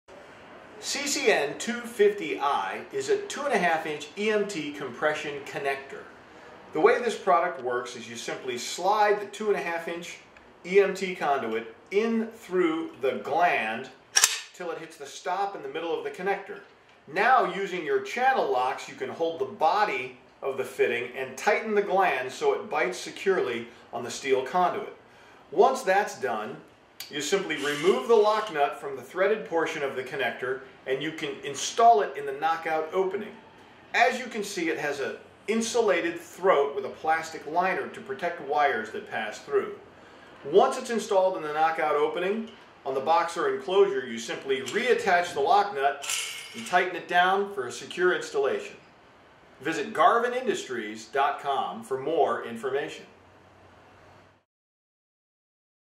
Compression Connectors: 2-1/2" Insulated Zinc Plated Compression Connector CCN250I
- 2-1/2" insulated compression connectors are used to bond 2-1/2" EMT thinwall conduits to electrical junction boxes or enclosures. Their plastic throat liner ensures that electrical wires and conductors do not get nicked or skinned by sharp metal edges. Garvin Industries' insulated steel compression connectors provide concrete tight connections when taped, and are rated for electrical applications above 600 volts. Their heavy gauge steel construction maintains mechanical protection and solid grounding of the conduit to the designated electrical junction box or enclosure. Compression connectors are also zinc electroplated for excellent corrosion protection in damp locations. Garvin Industries' labor saving design allows the electrician to quickly tighten these connectors on EMT conduits.